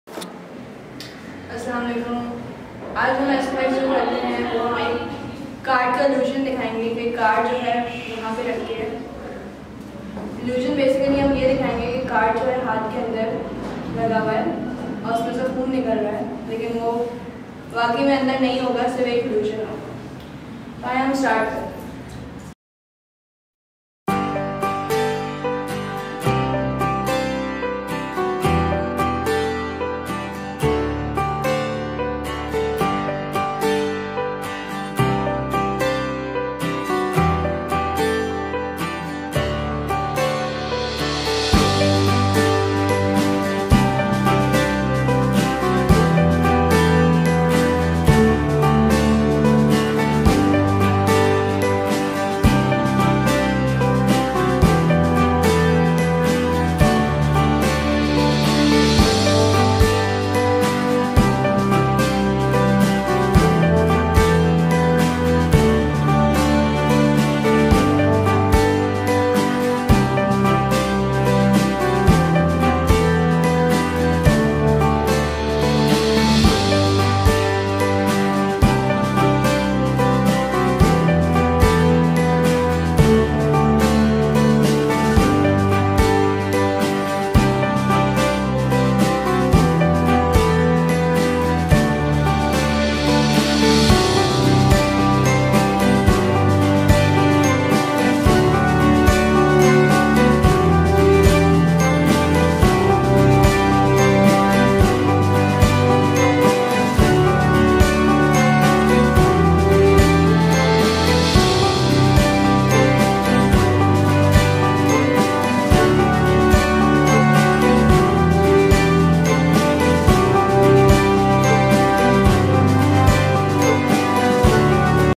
North City School of Art | Card on Hand | SFX Art
Card on Hand effect done by North City School of Art students